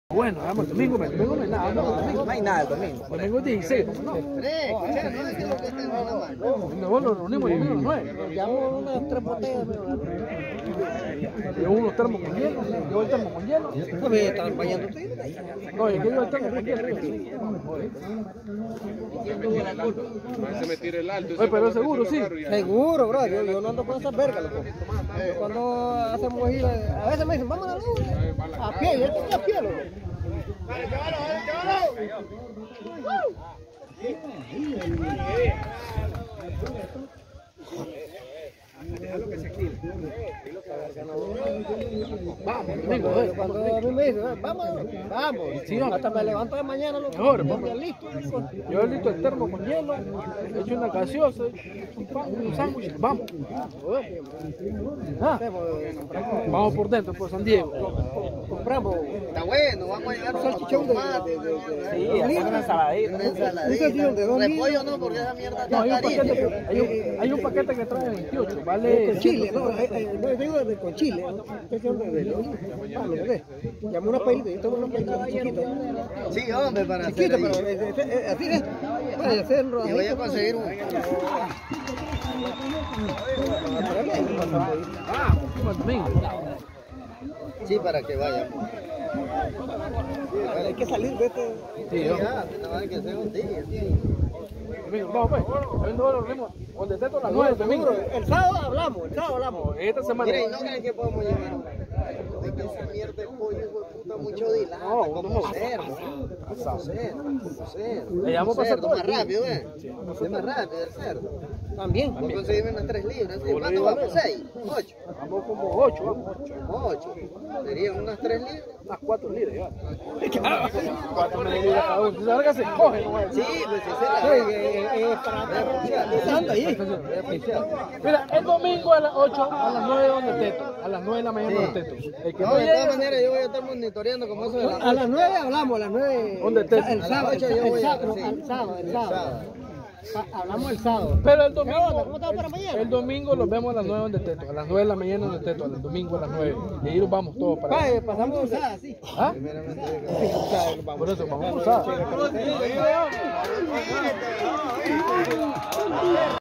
Final Beisbol Mayor ´´A´´ Liga G.D.E - Diriomo - Granada - 9/01/2022
Juego numero 6 de la serie final
ASTROS DE DIRIOMO GANADOS - 2
BRAVOS DE DIRIOMO GANADOS - 3
 Serie de 7 juego a ganar 4

El ganador jugara la interliga en busca del boleto que lleve al departamental mayor A. Con sede esta vez en la ciudad de los brujos en Diriomo, Granada.

CCD:@305